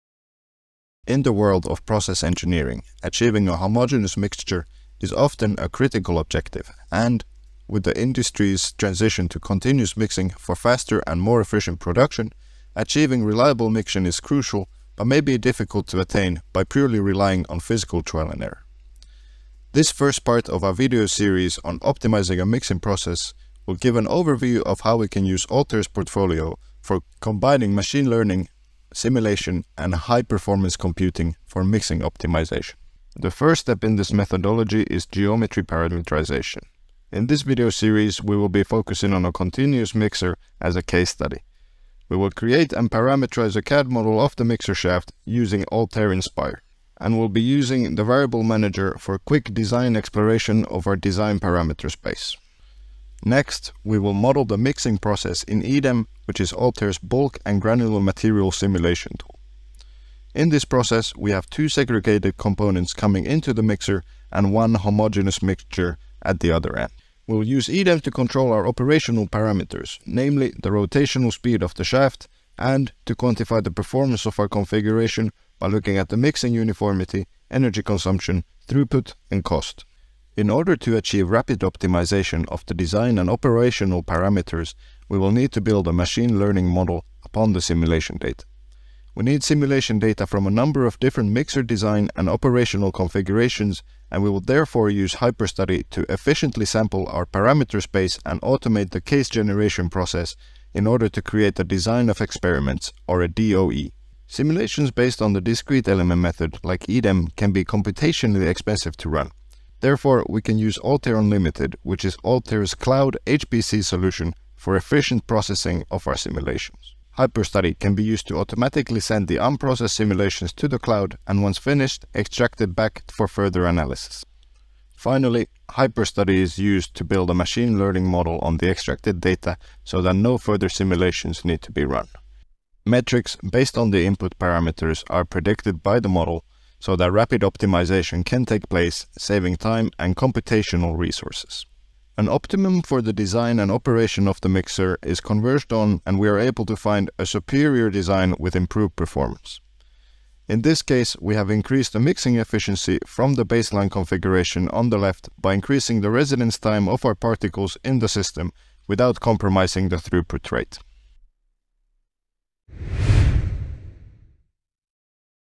How to Optimize a Continuous Powder Mixer with Altair tools – Part 1 Overview
Learn how to use the Altair portfolio of tools to optimize the design and operation of a continuous bulk solids mixer. In this 4-video series you'll learn how to:

1. Parameterize a mixer CAD model in Altair Inspire.
2. Simulate the mixing process with Altair EDEM, a physics-based simulation software for modelling granular materials.
3. Apply machine learning and automation with Altair HyperStudy, a design exploration software that can explore and optimize design performance and operation.

By combining these Altair tools, you'll be able to perform efficient virtual optimization of mixer design and operation.  Let’s start!